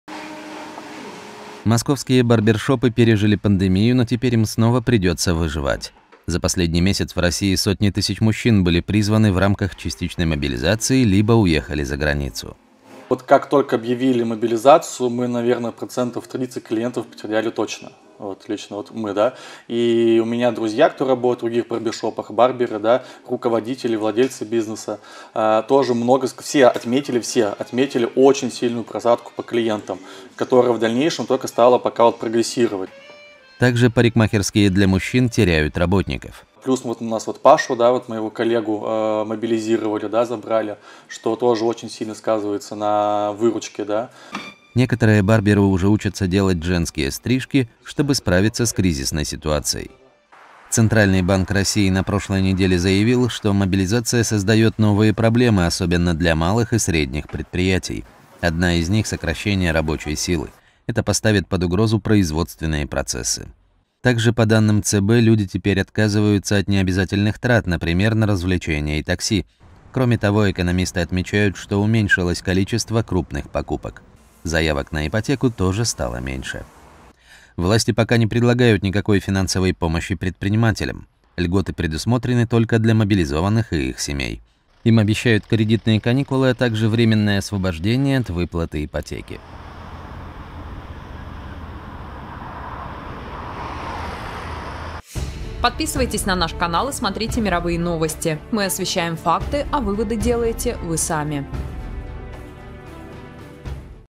Мобилизация отнимает клиентов у барбершопов
Мобилизация принесла новые трудности российским предпринимателям. Так, барбершопы в Москве уже потеряли треть клиентов.